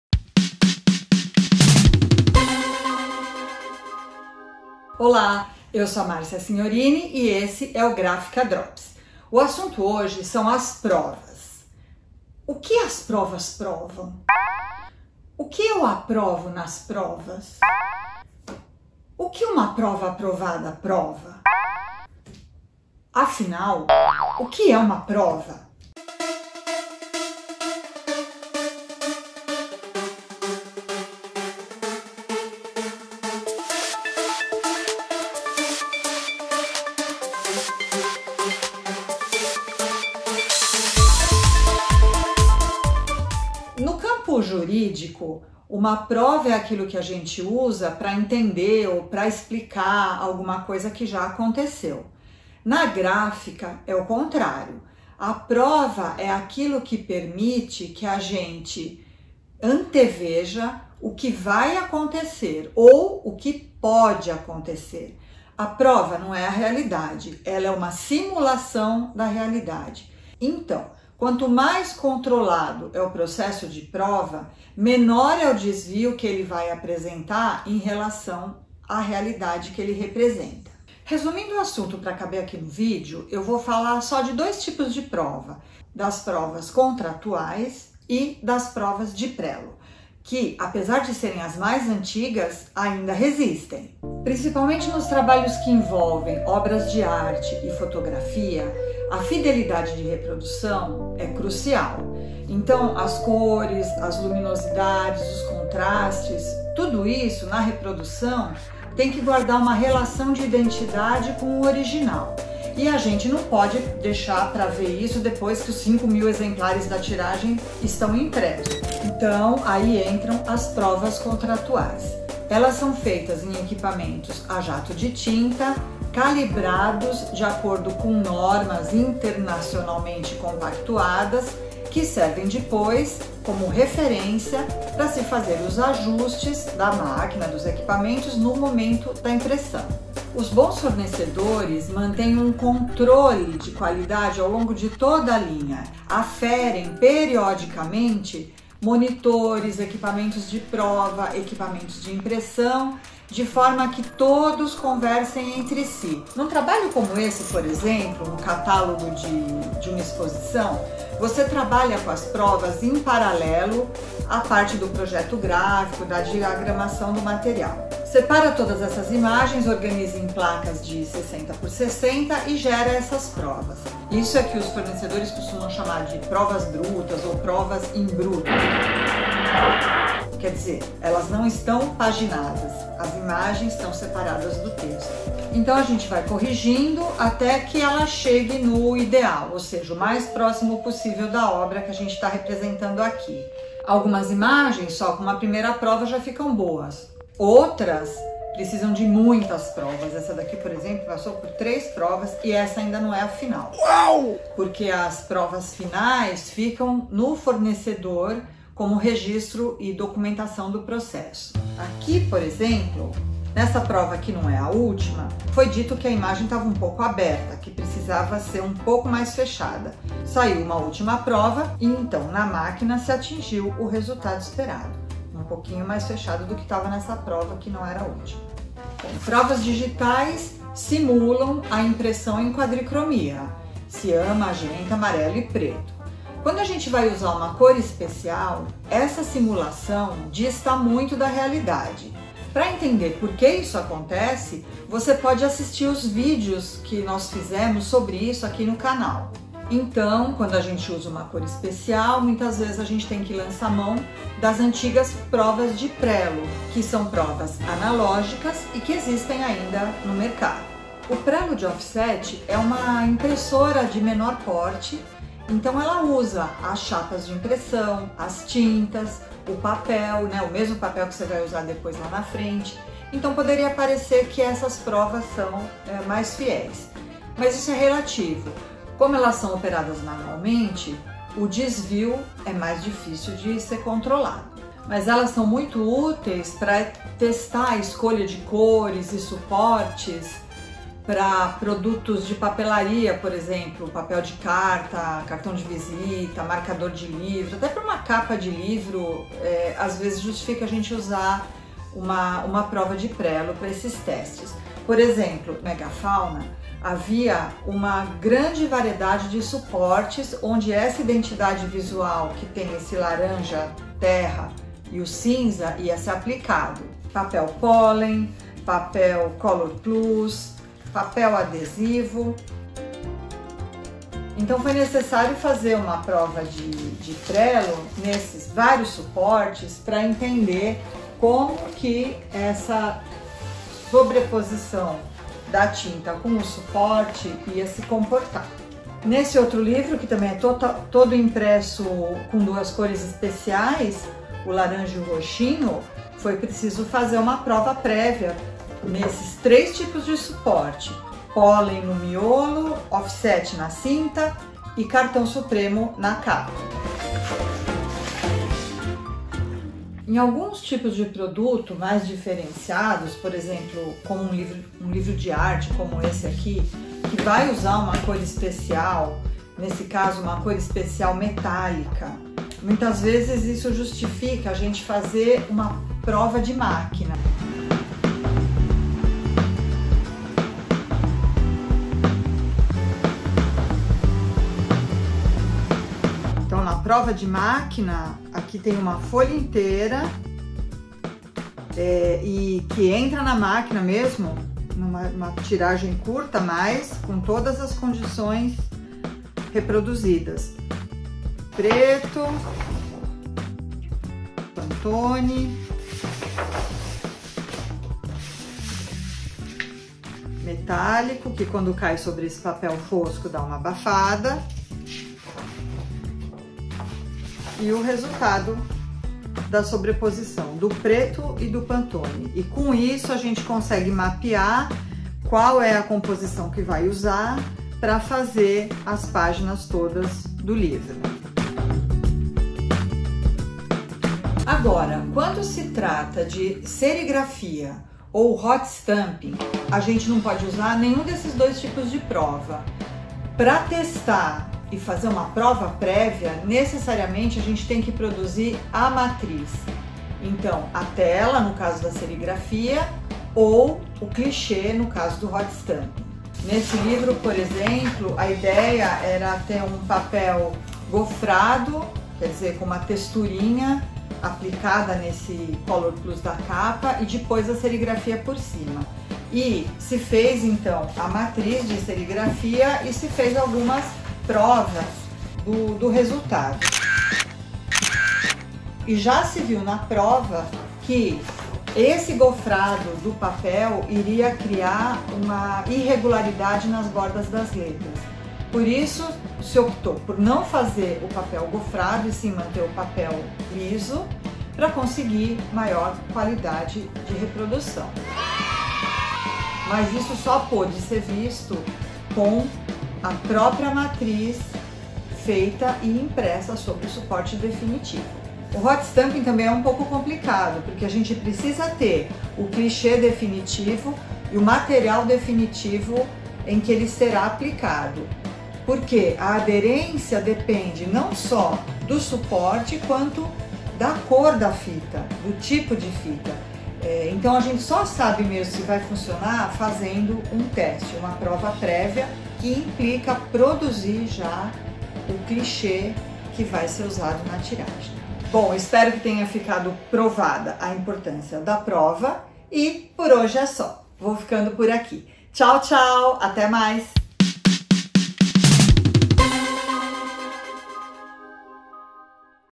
As provas e seus segredos
As provas são  muito importantes para controlar o processo de produção gráfica; conheça nesse vídeo dois tipos de prova muito utilizados atualmente: as provas contratuais e as provas de prelo.

Livros mostrados no video
Fayga Ostrower: Imaginação tangível. Catálogo de exposição Pinacoteca de São Paulo, 2021
O livro da barista. Coffee Lab, 2021
Erika Verzutti, a indisciplina da escultura. MASP, 2021

Links relacionados ao assunto
Equipamentos para impressão de provas contratuais
Epson

Fornecedor de provas de prelo em São Paulo:
Prelograph